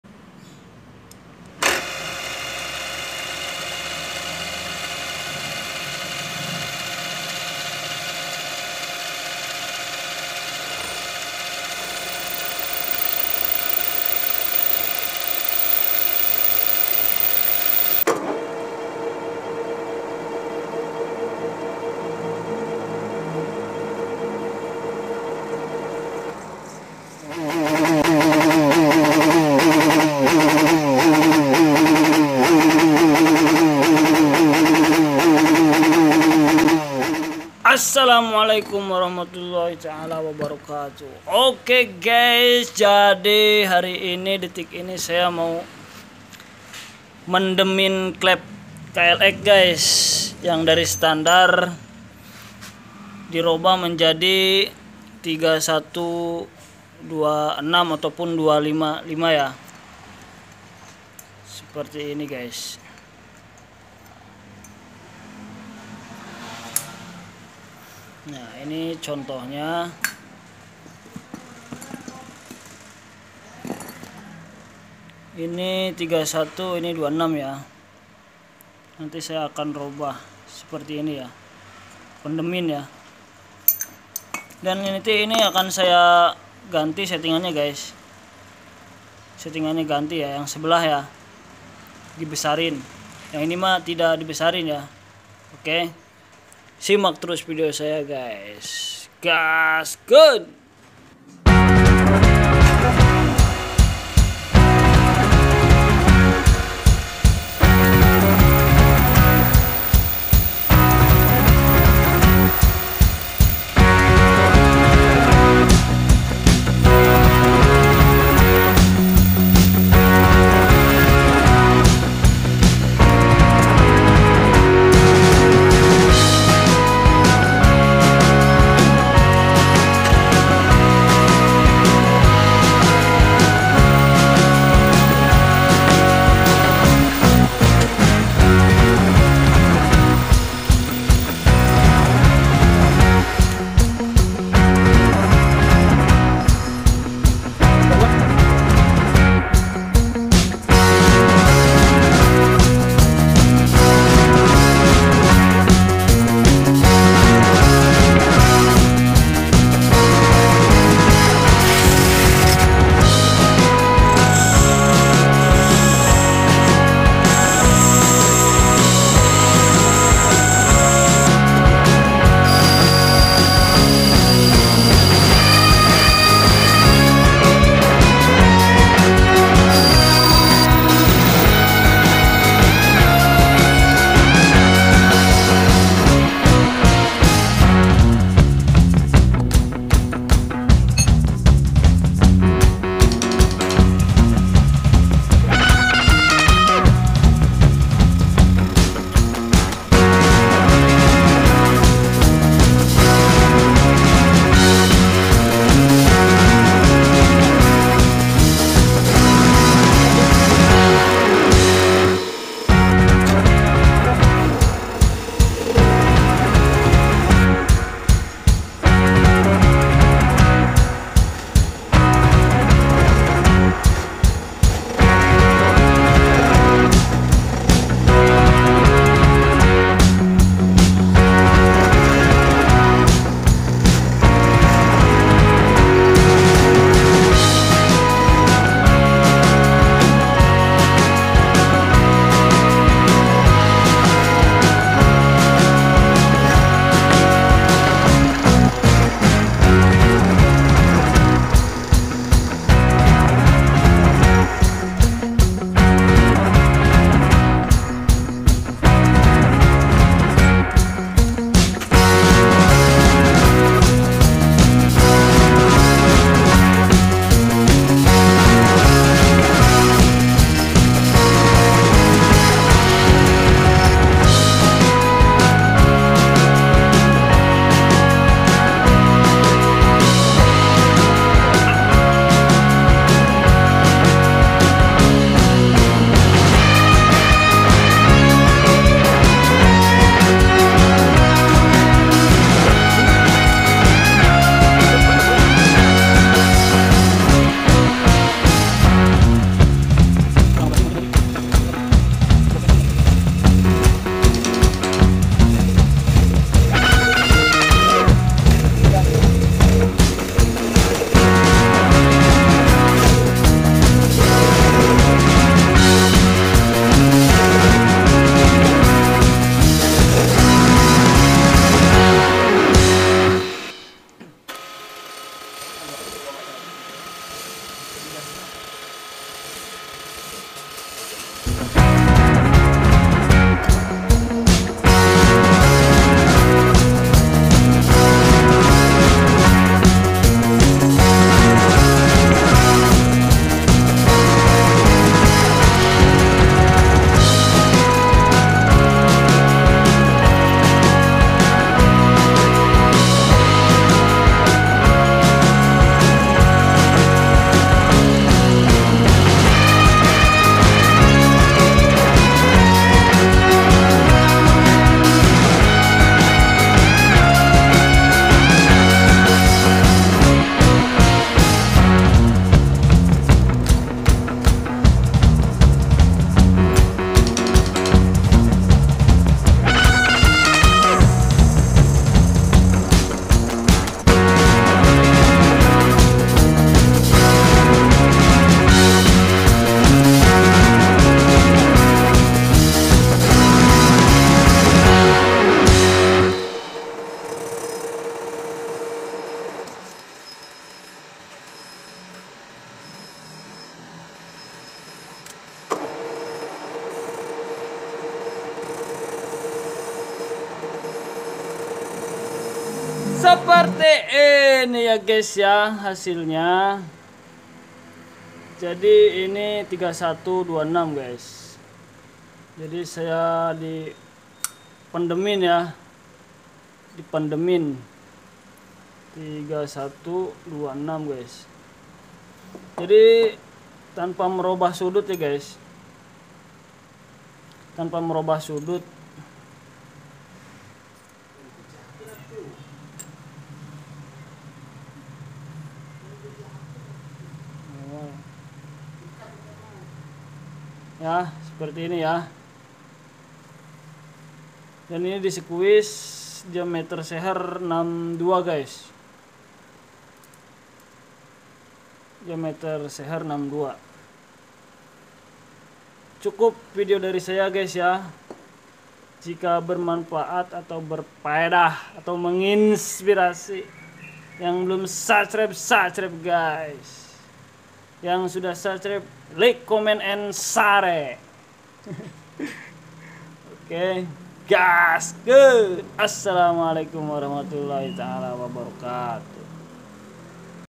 proses pendem head klx 26 31
cara saya merobah head klx spek trabas . menggunakan klep 31-26